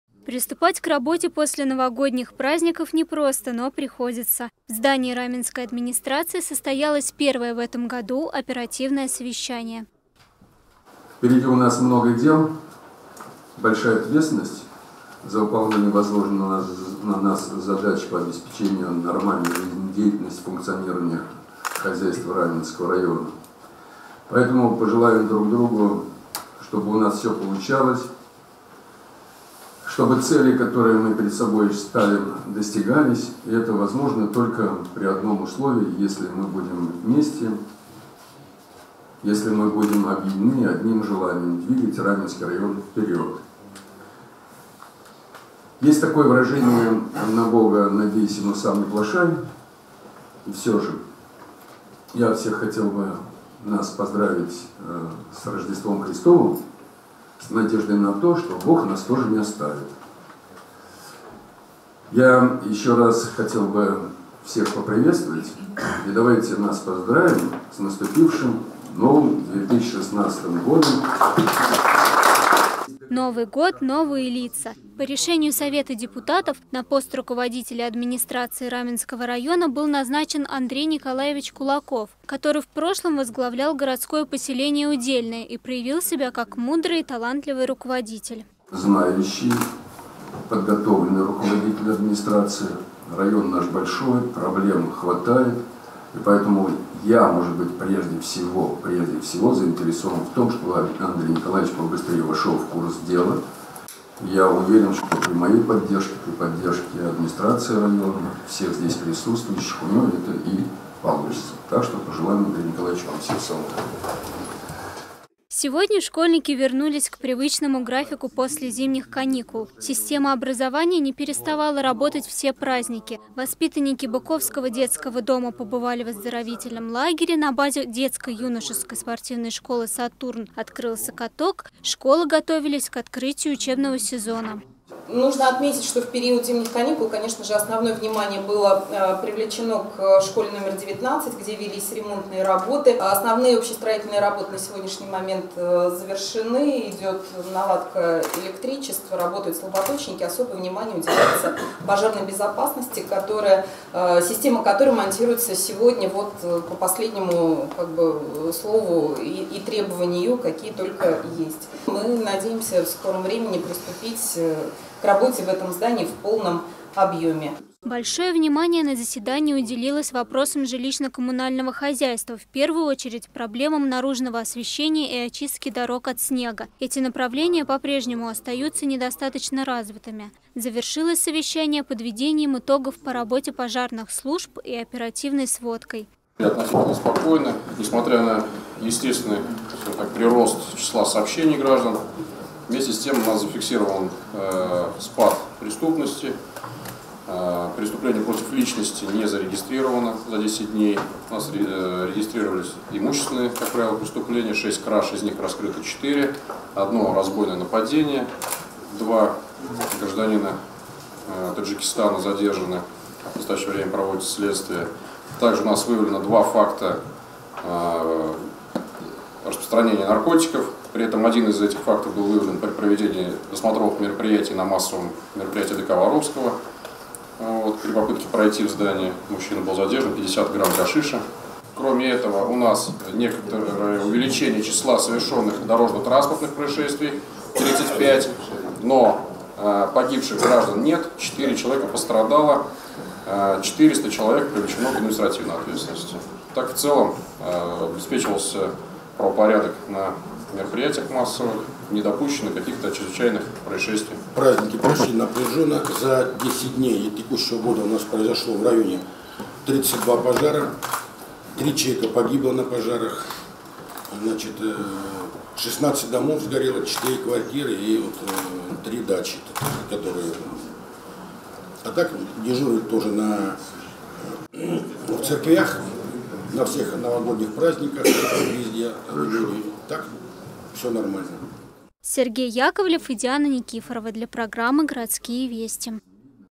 11 1
Новогодняя шумиха, праздничные гуляния и уютные семейные посиделки остались позади, впереди – год усиленной работы со своими трудностями и победами. В Раменской администрации трудовой год начался с традиционного оперативного совещания.